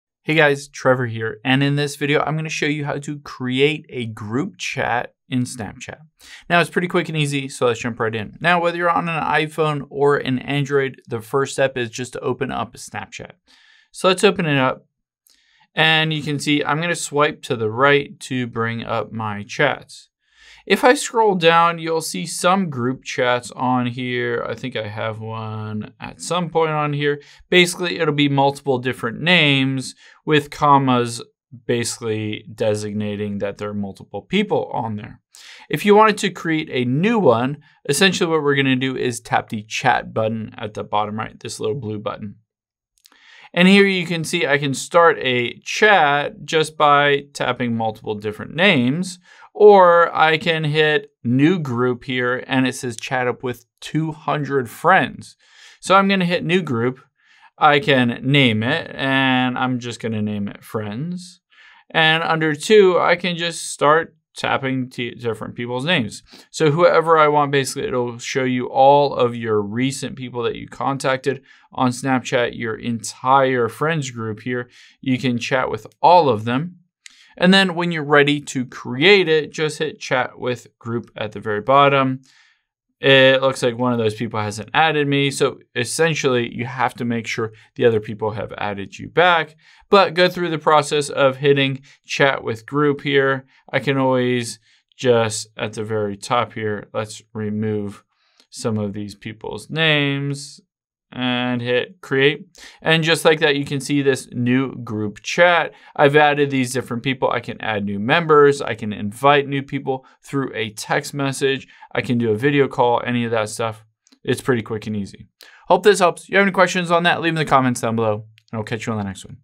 How To Create A Group Chat In Snapchat
Here's how you can start your own group chat on Snapchat if there are several or dozens of people you want to talk to in your class, etc.

Cool iPhone Facts:
- Every single iPhone ad shows the time as exactly 9:41